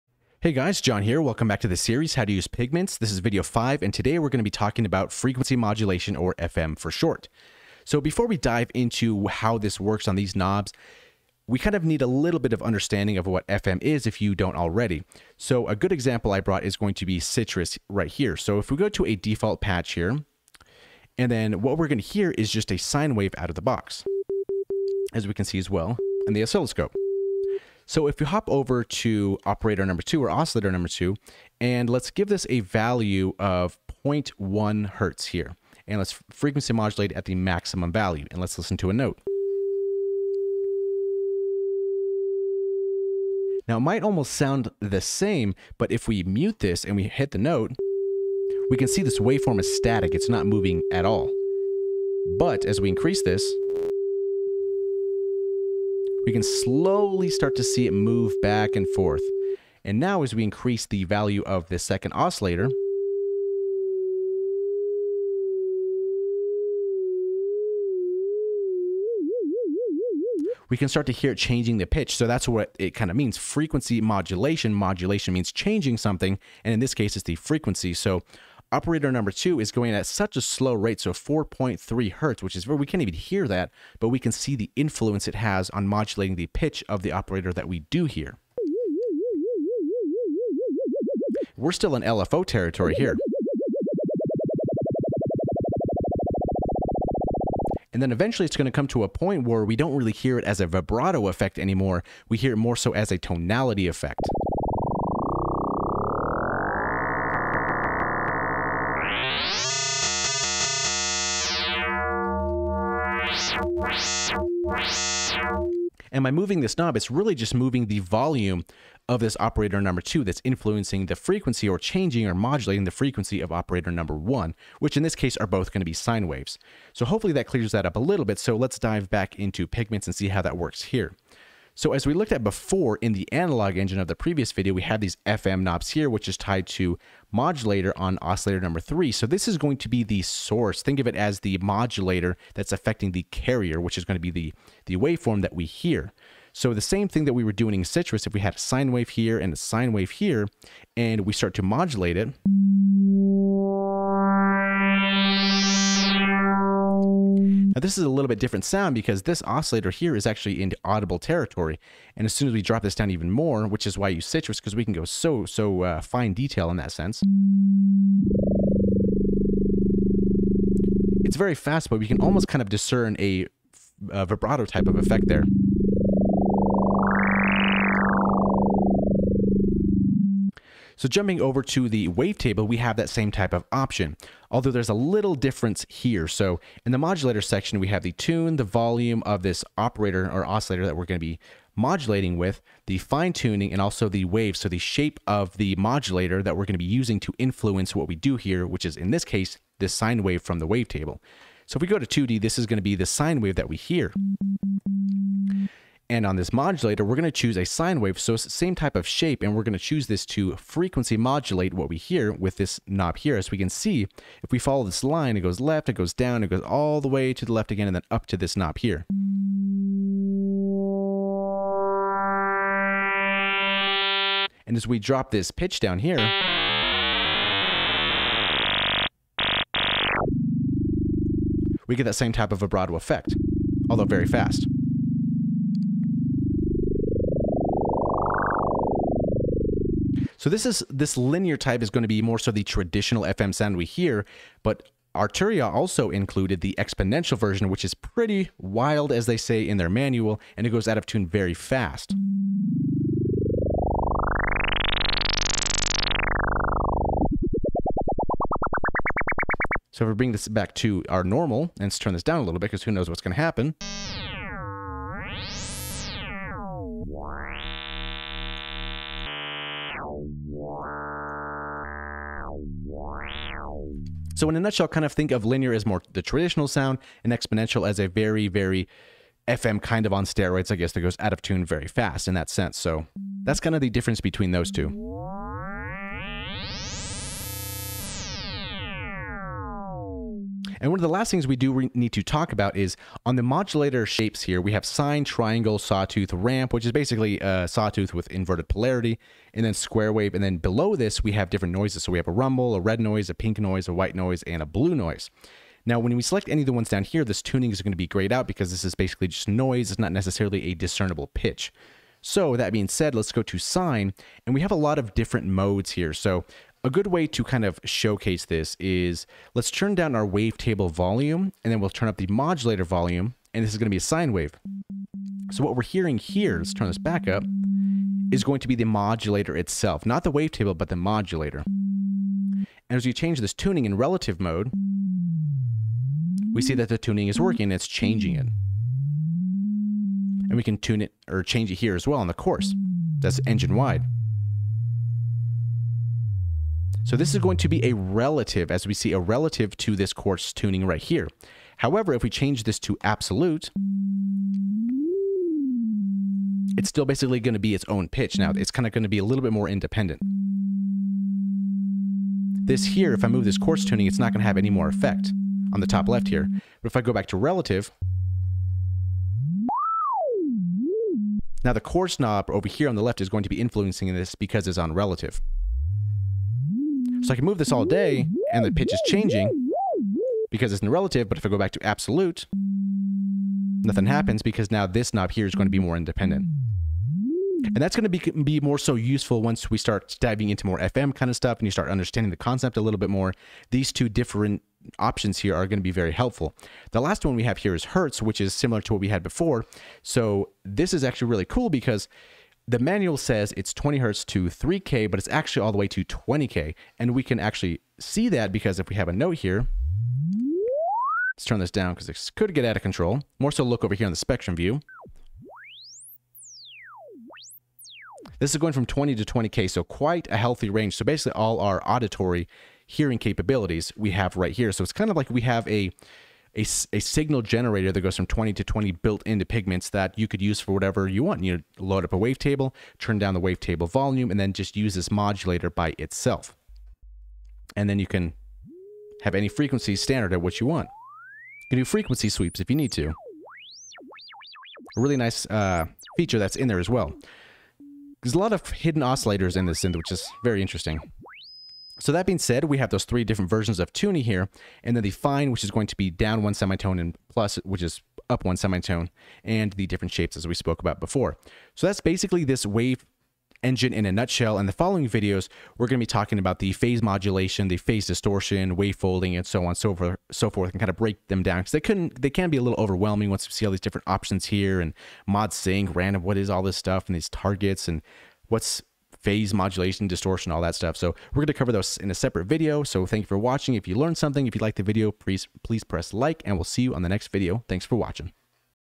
FM Frequency Modulation PART 5 | Arturia Pigments 3.5 Tutorial
This is part five of the tutorial series 'How to use Arturia Pigments'.
In this video we discuss what FM (frequency modulation) is, what it does and how it is applied in pigments in the wavetable and analog engines.
FM in pigments offers a few different options and makes FM unique to this synth!

My favorite vst plugins:
Synths:
Arturia Pigments 4

Effects:
Arturia FX Collection 3